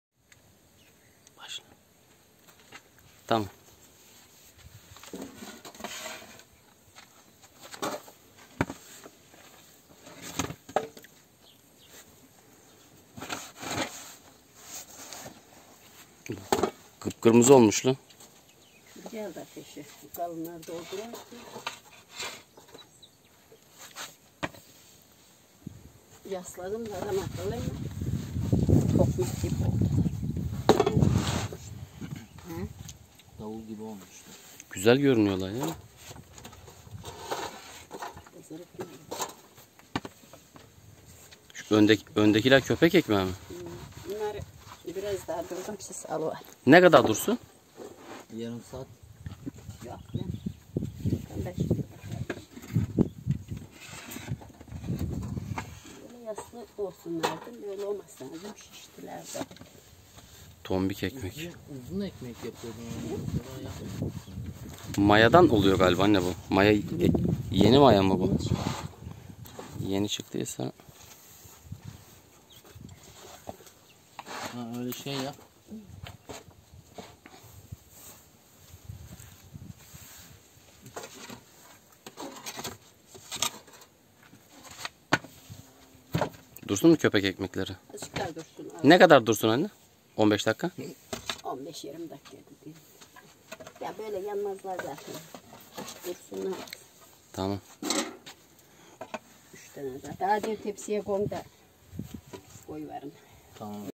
Kara fırında meşe odununda taptaze, kıpkırmızı köy ekmeği+tereyağı=😁
Tereyağıyla mükemmel oluyor 😁